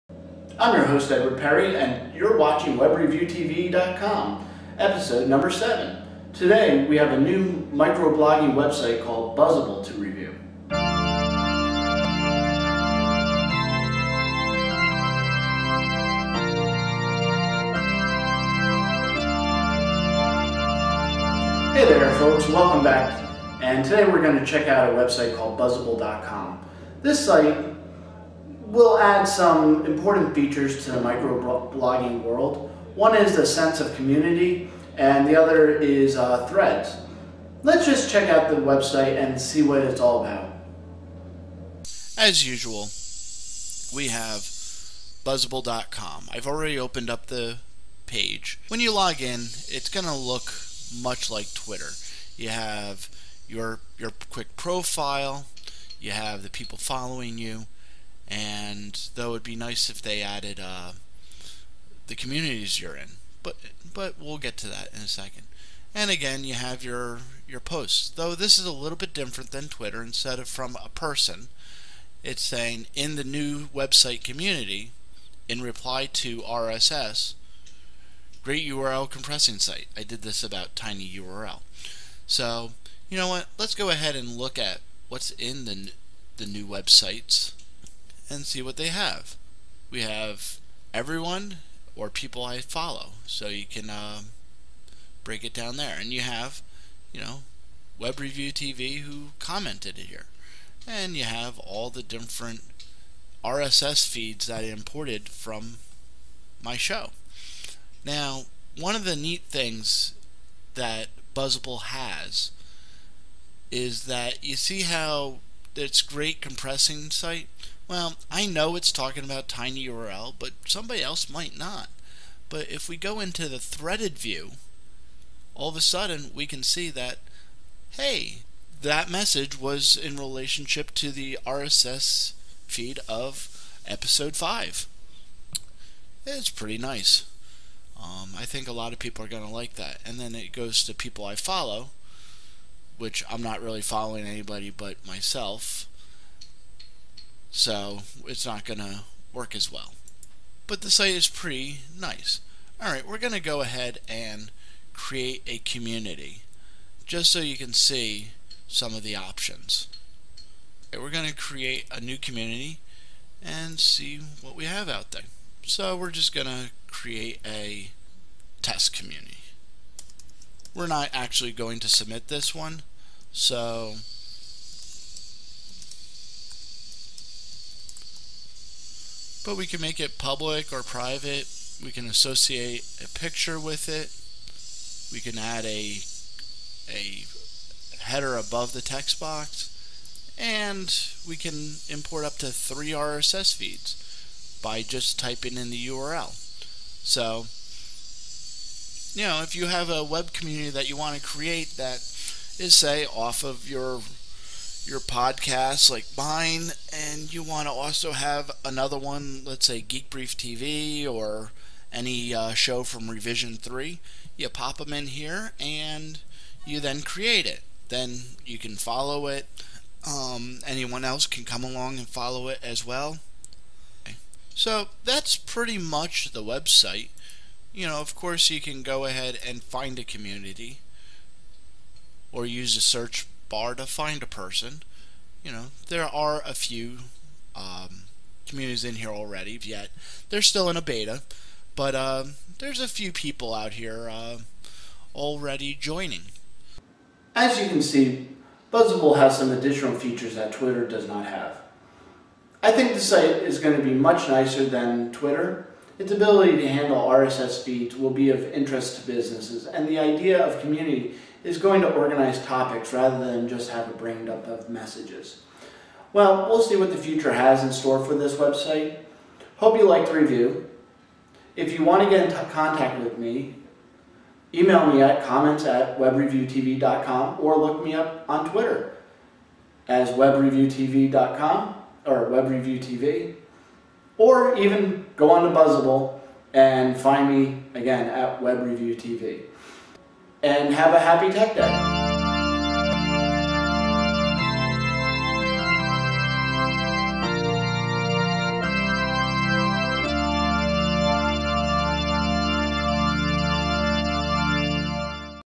WRTV E7 Buzzable.com
Today we are reviewing a micro-bloging website called Buzzable.com